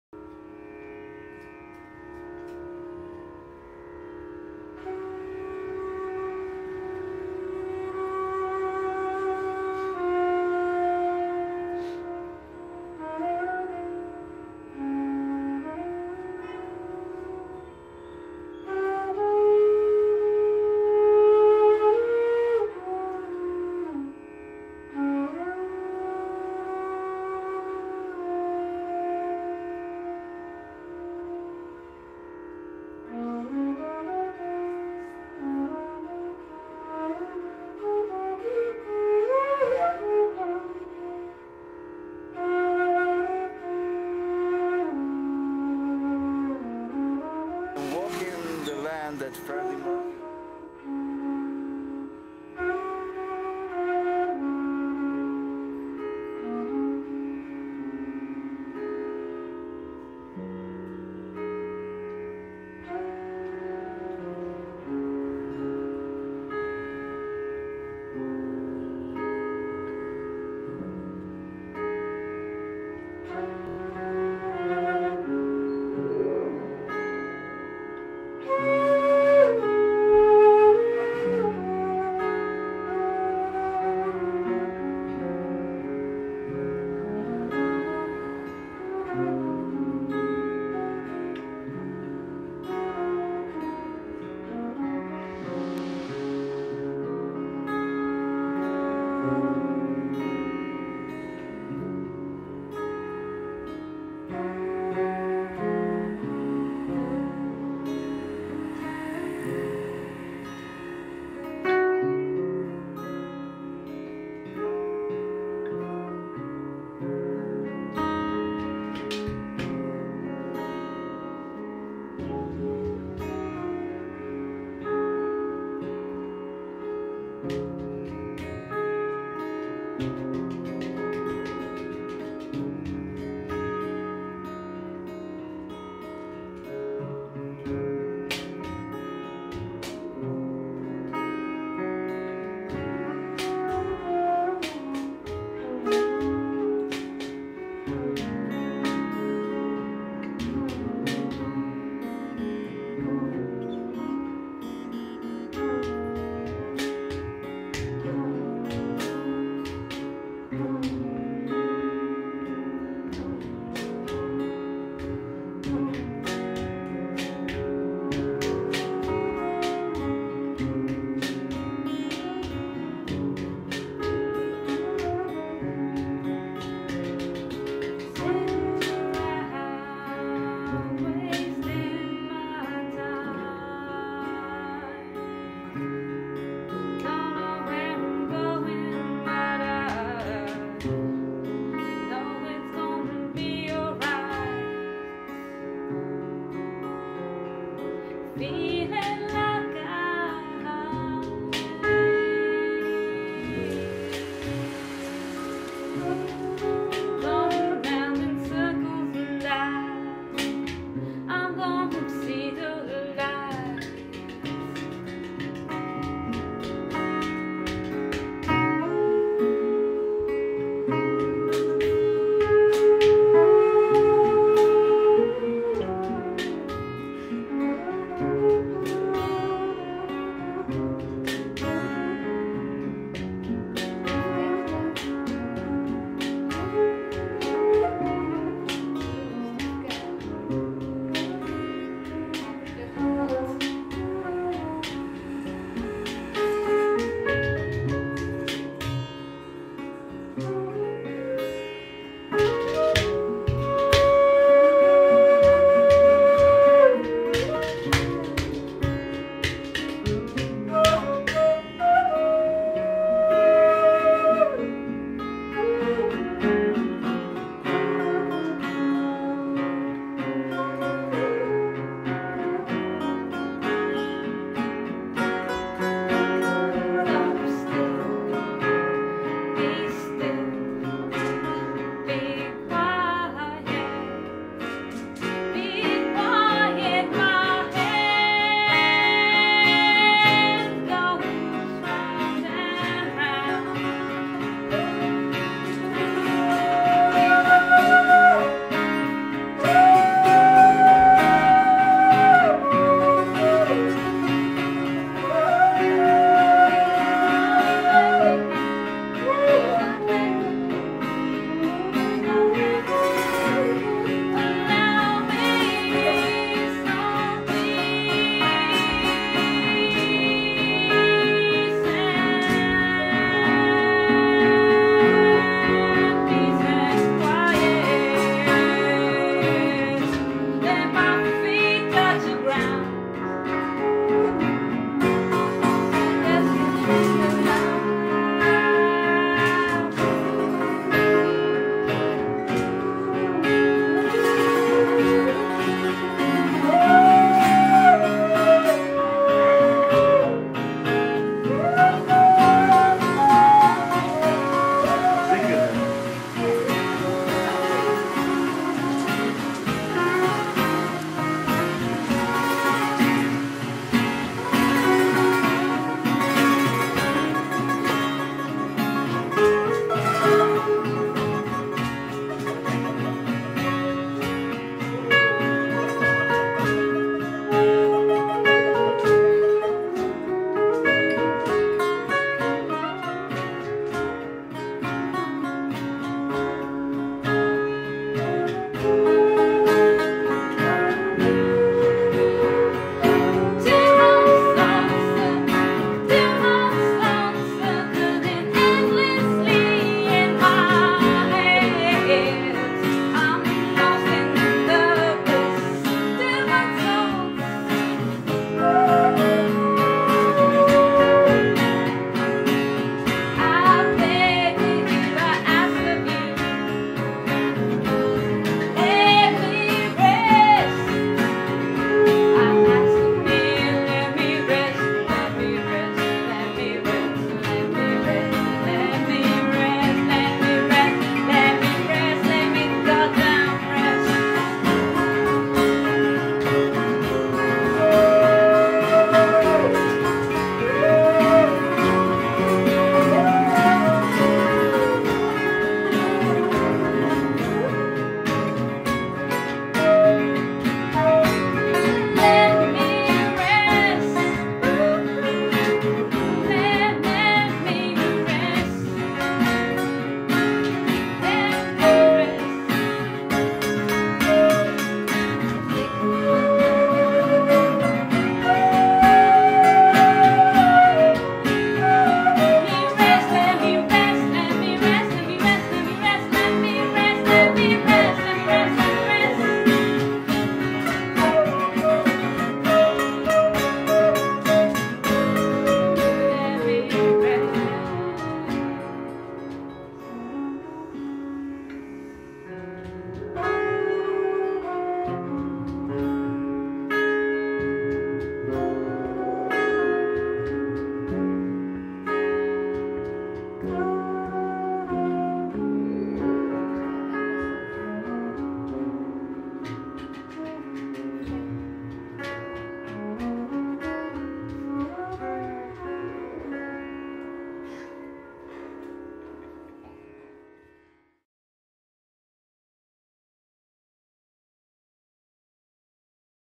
Rebecca and Micky in India - ReMi
A journey set to a jam that i played in india.